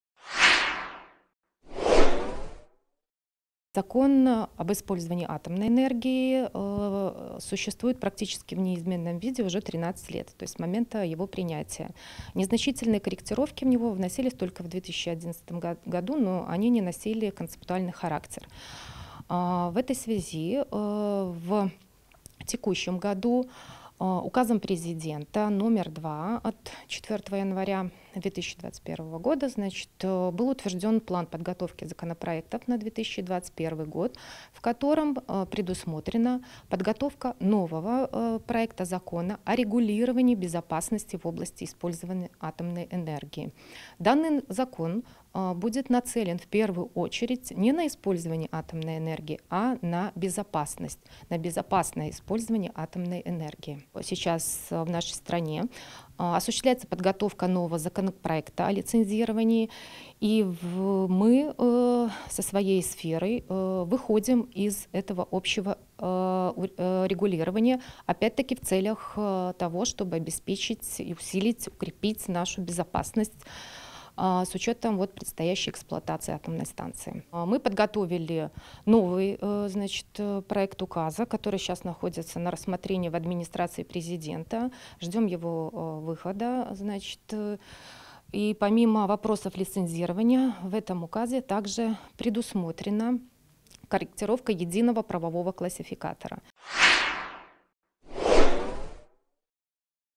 В Беларуси готовят проект закона о регулировании безопасности при использовании атомной энергии
В Беларуси разрабатывают проект закона о регулировании безопасности при использовании атомной энергии. Об этом сообщила начальник сектора правового обеспечения Департамента по ядерной и радиационной безопасности МЧС (Госатомнадзора) Елена Жигалко во время брифинга в пресс-центре БЕЛТА.